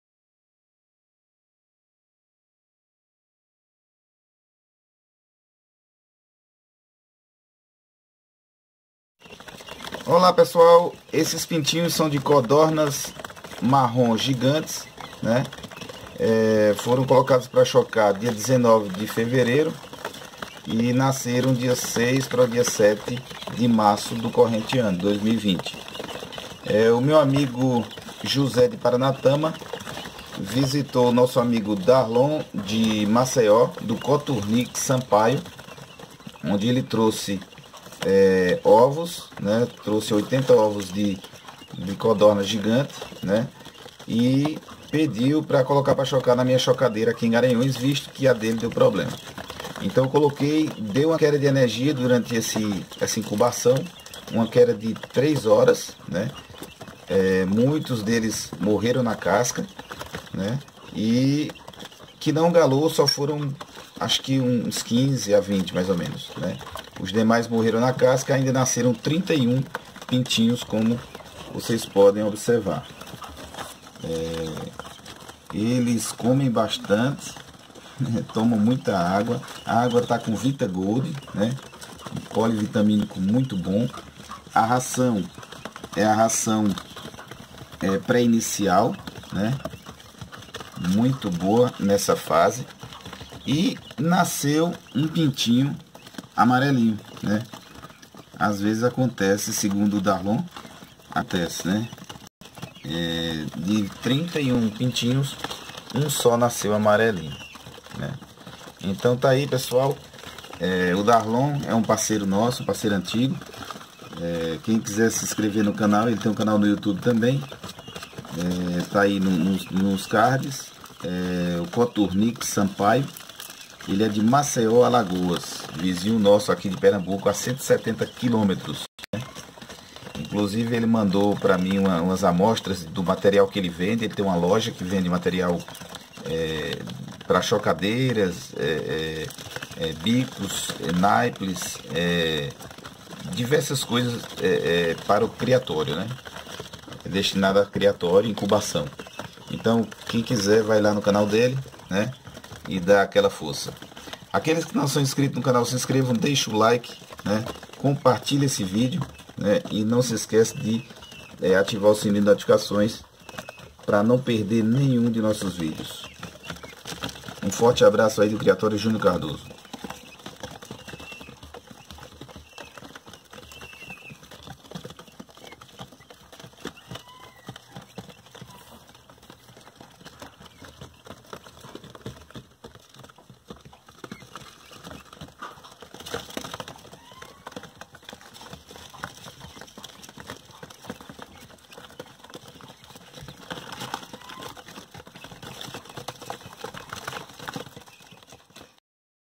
Veja os Pintinhos de Codornas Gigantes advindas de Ovos Galados do Coturnix Sampaio
Algumas de nossas aves:
Aves e ovos galados a venda.

Contatos:
Whatszaps Tim (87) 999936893 e Vivo (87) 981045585.